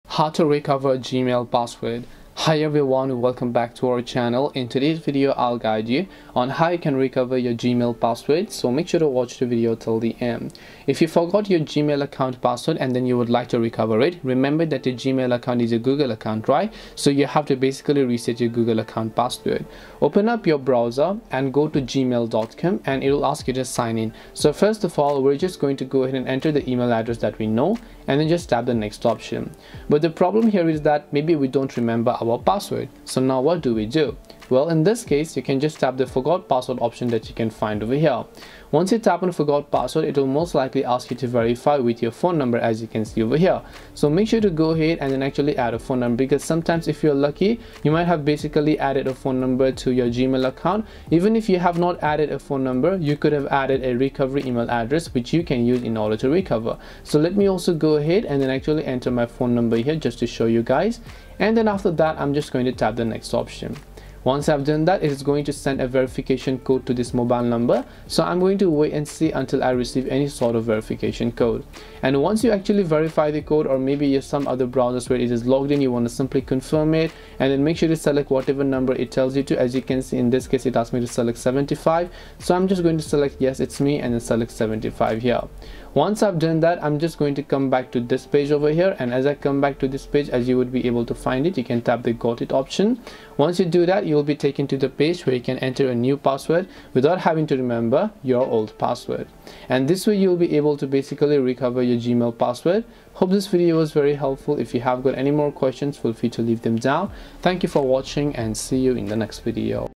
How to Recover Gmail Password !
This video guides you through an easy step-by-step process to recover your Gmail account password. So make sure to watch this video till the end.

~ Chapters:
0:00 Introduction
0:10 Recover Gmail Password
1:39 Conclusion